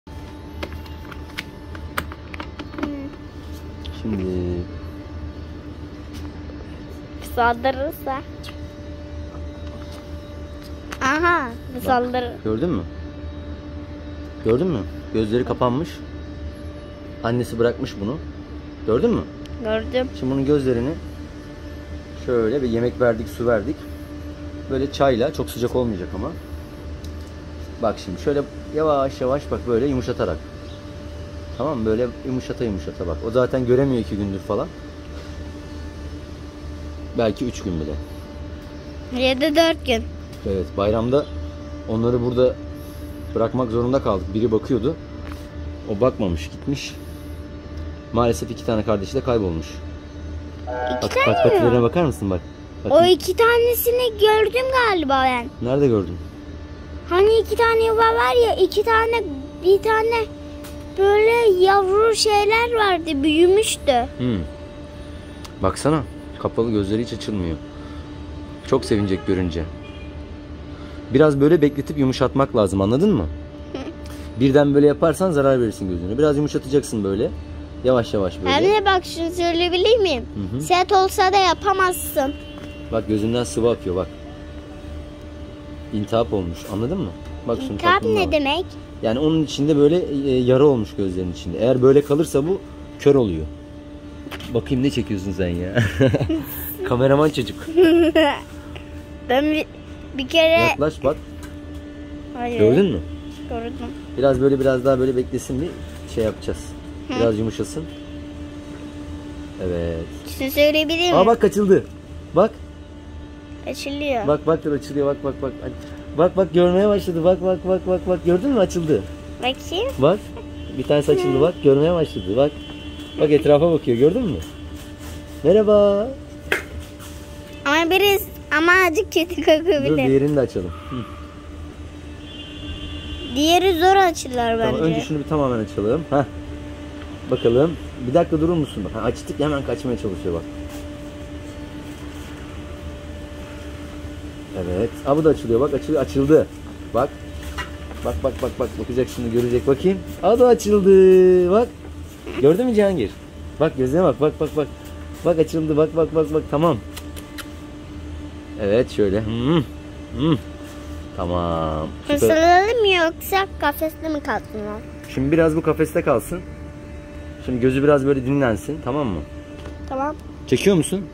ÇAY POSASI İLE GÖZ AÇMA
annesi tarafından terk edilmiş gözleri kapalı yavru kedilerin çay posası ile gözlerini açmaya çalıştım